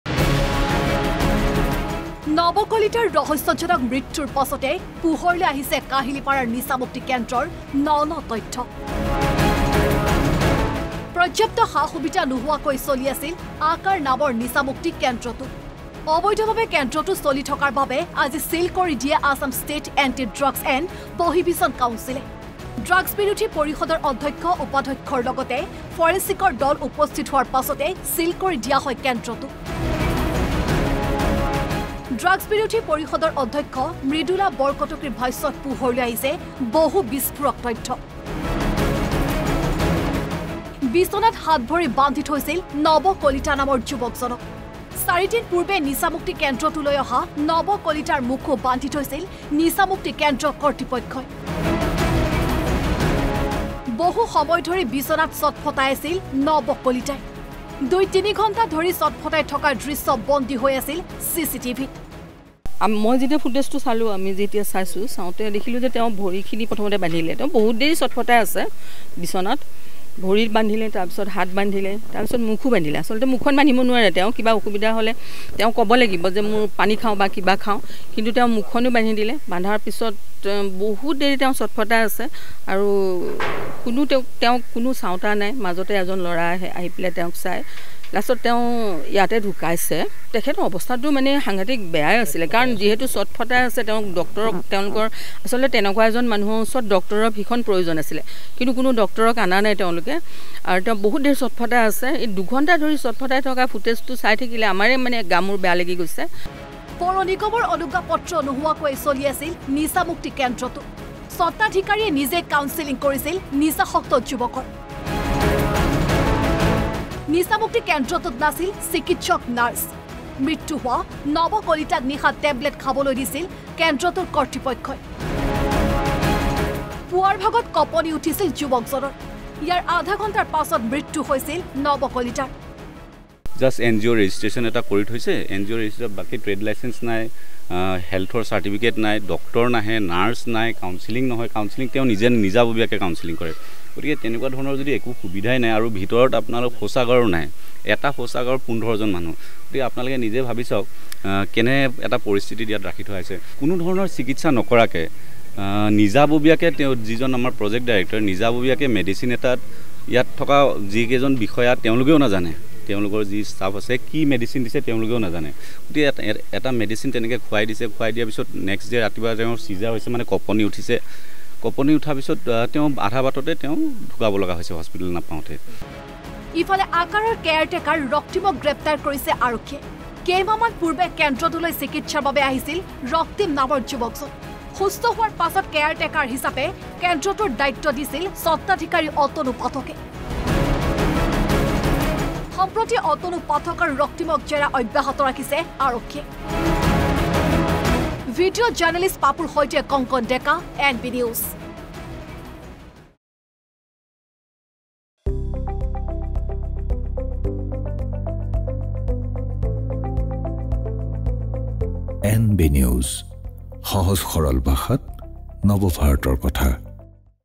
CCTV ফুটেজত বন্দী নিচামুক্তি কেন্দ্ৰত যুৱকৰ মৃত্যুৰ ৰহস্য
🔴পোহৰলৈ আহিছে কাহিলিপাৰাৰ নিচামুক্তি কেন্দ্ৰৰ ন ন তথ্য
🔴CCTV ফুটেজত বন্দী নৱ কলিতাৰ মৃত্যুৰ ৰহস্য